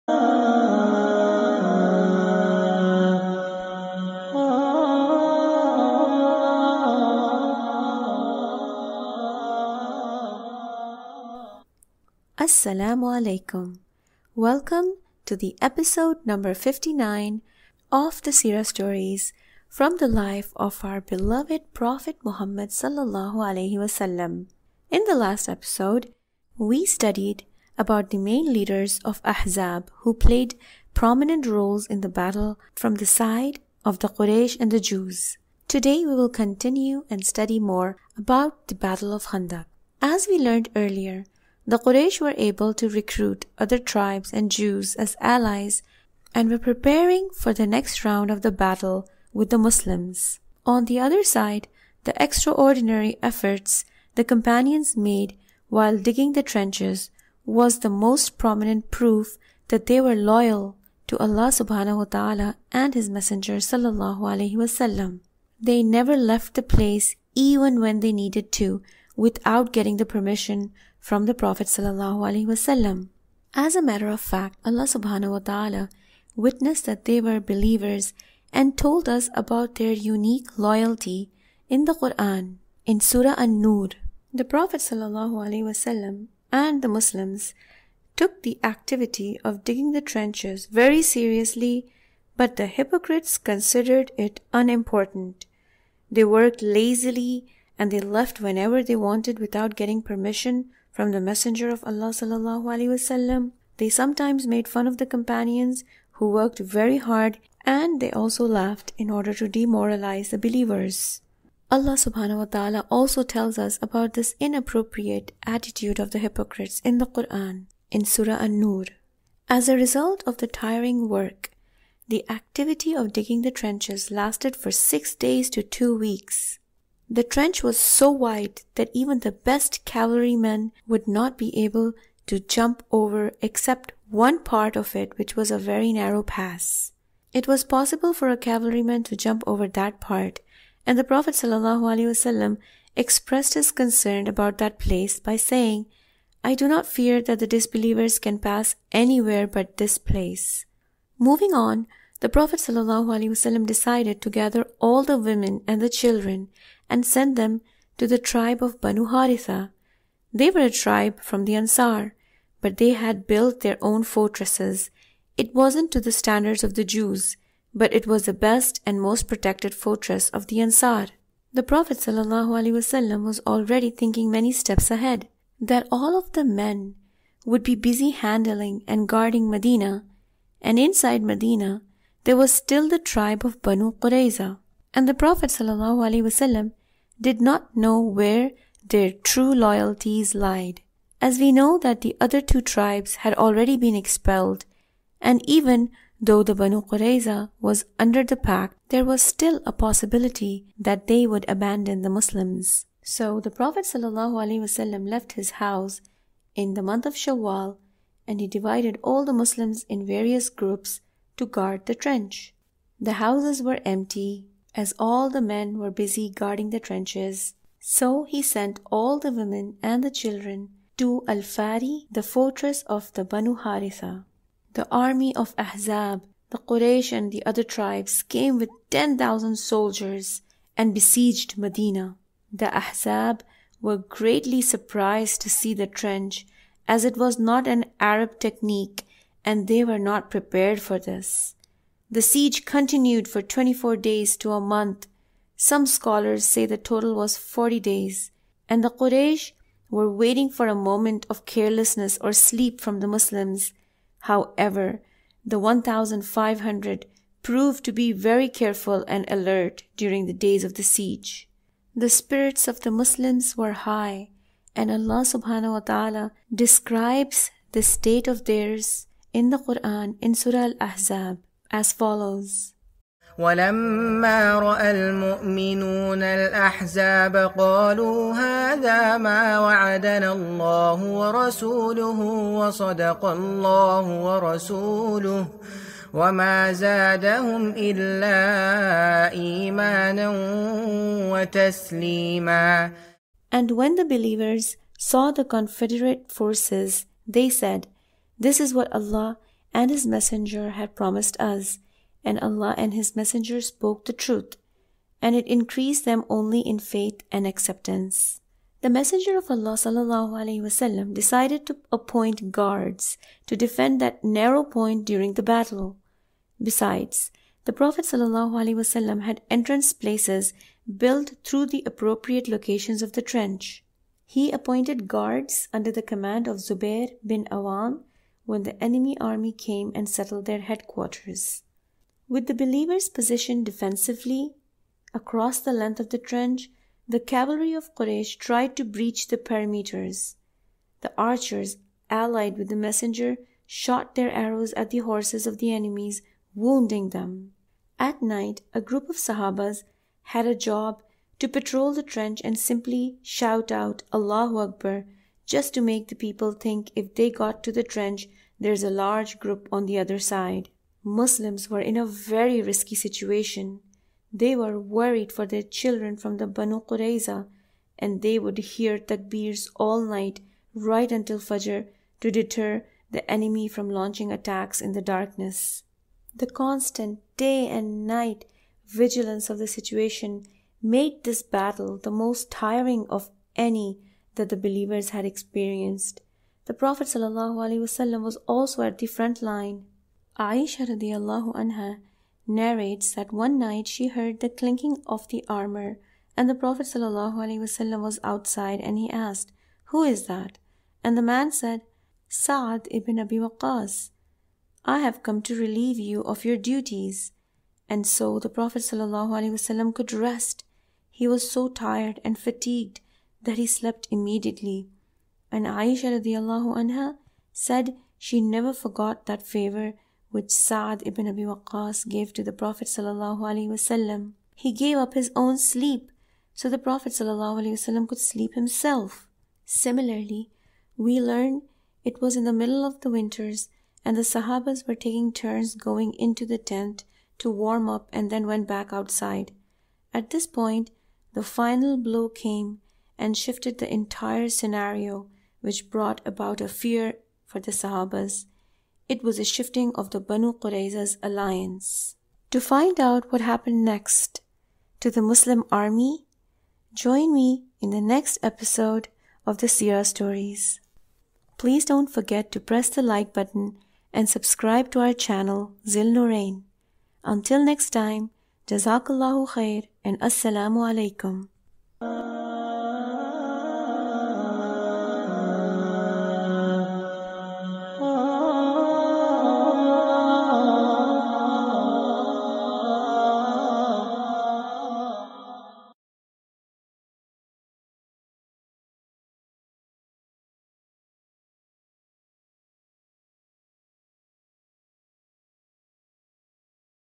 Seerah Series for Kids | Episode 59 | Events of Trench | Part 4
The Events of Trench was unique because it was not fought traditionally. It was a vital and decisive battle against Quresh and needed unique strategic tactics. How to fight against a much bigger and more powerful army in a decisive war required unique tactics.@ZillNoorainKids Seerah Series | Episode 59 | Events of Trench | Part 4